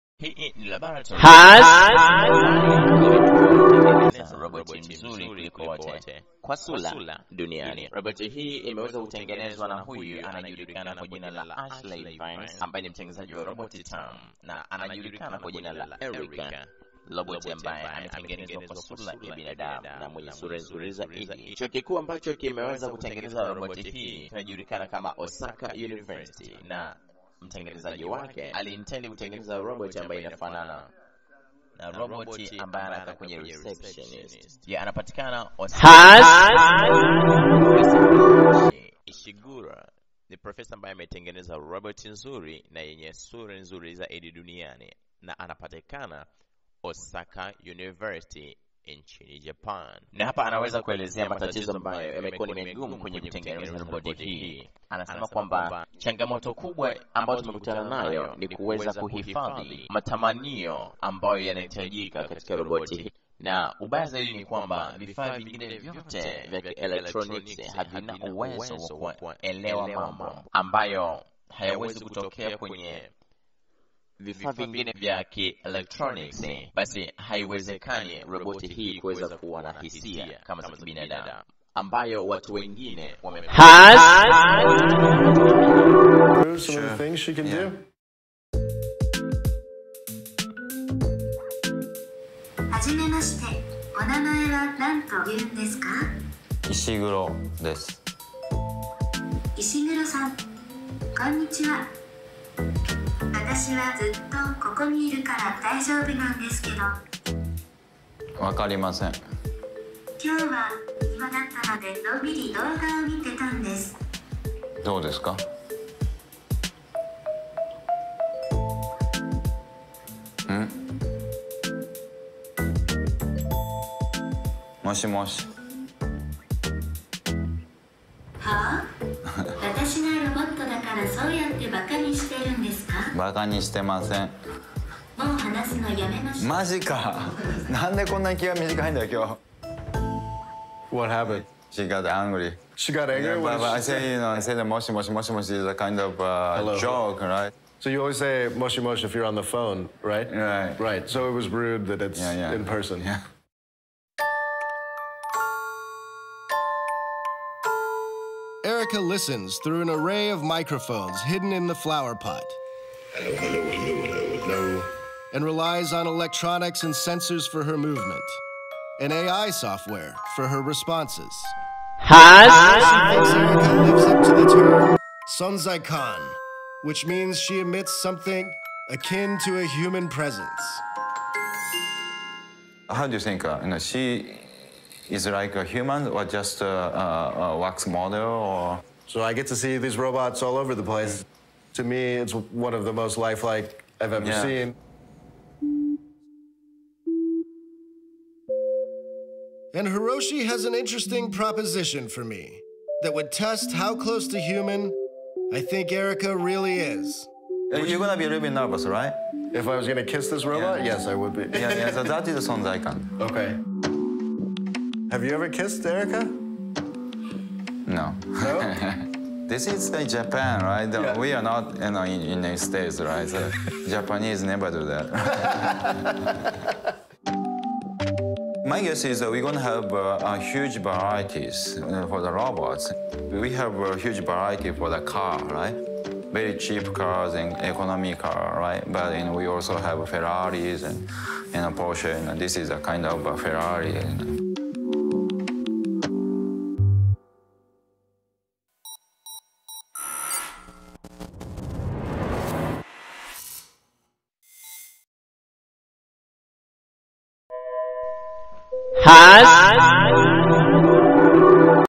roboti yenye sura nzuri zaidi duniani
roboti ifananayo na manamke mzuri zaidi duniani imetengenezwa nchini JAPANI